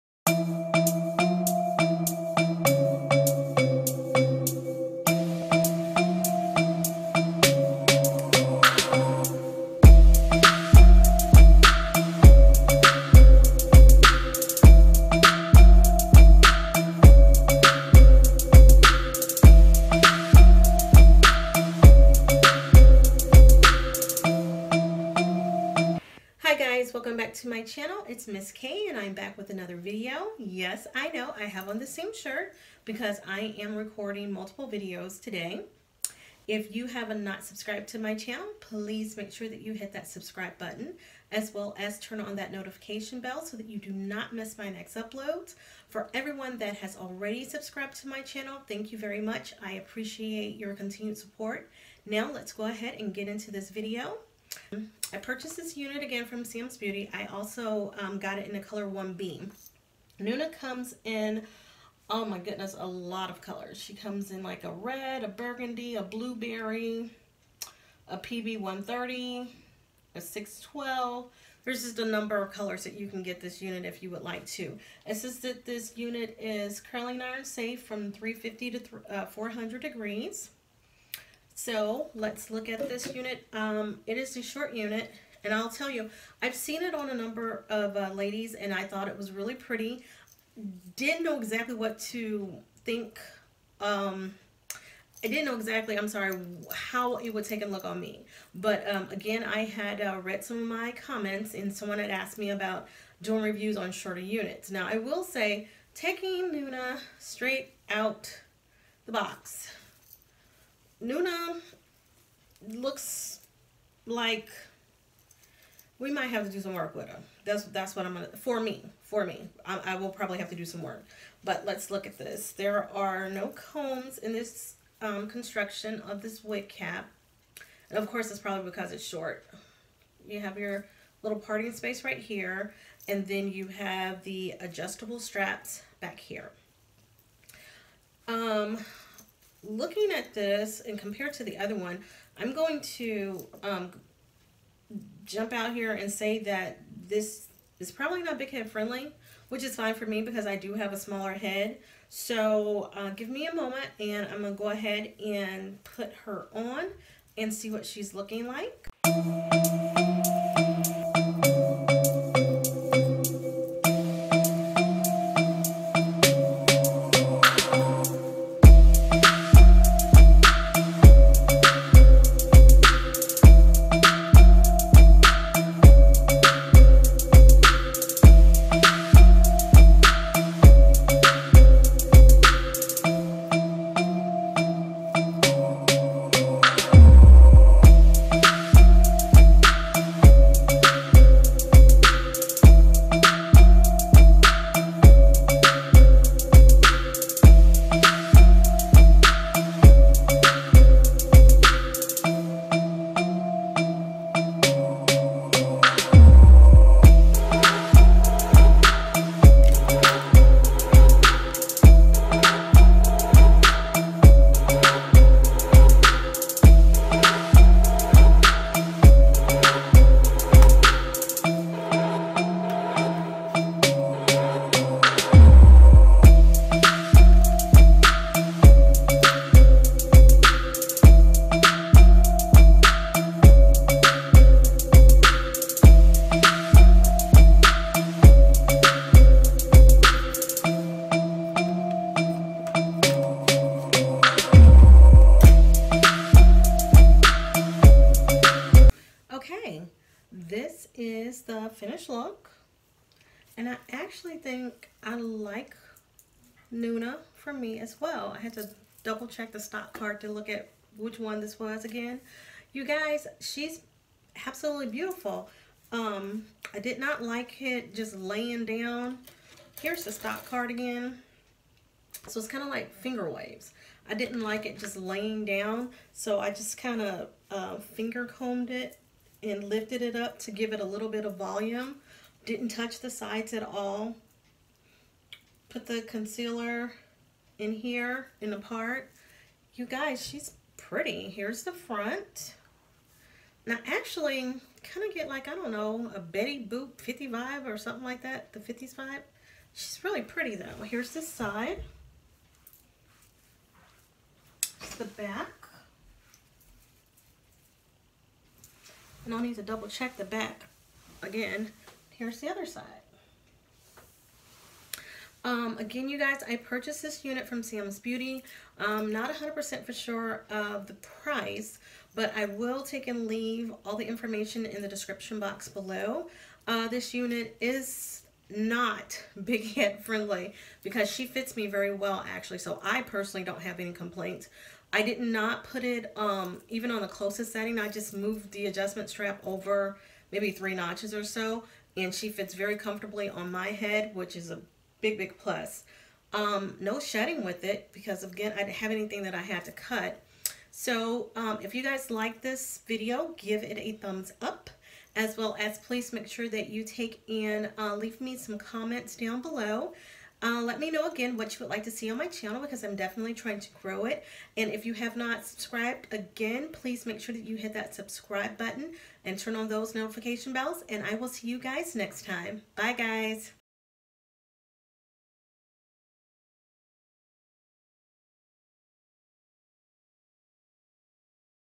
It's A Wig Nuna Wig Review| Must have wig for the Summer
I purchased this unit from SamsBeauty for under $17.99. I hope you enjoy this review.